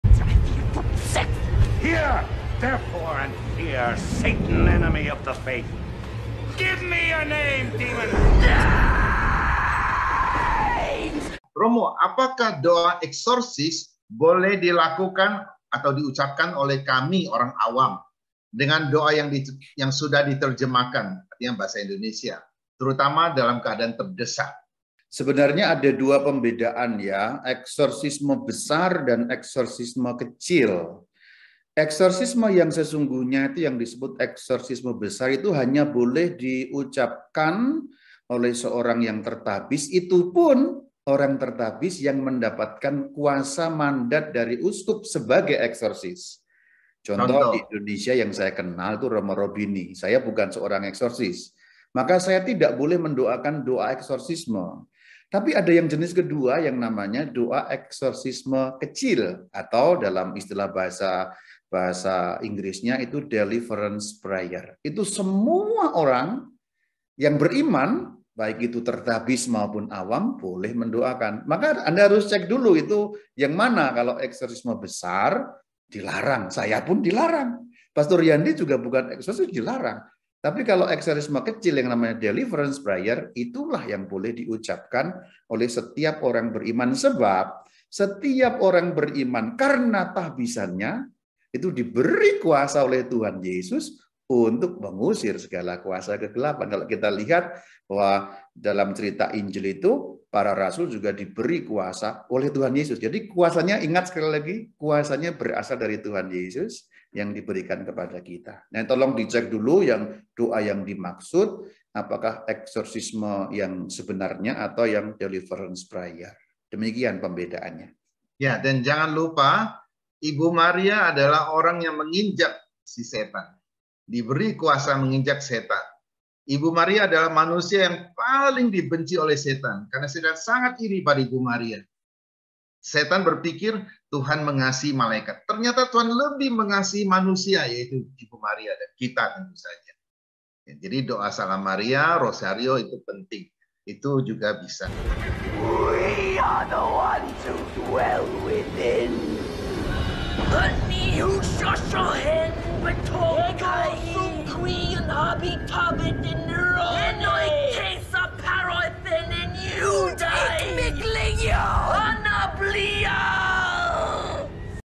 Exorcism dalam ajaran katolik
bersama Romo Yandi cdd dan Rama bayu

sumber : katkit chanel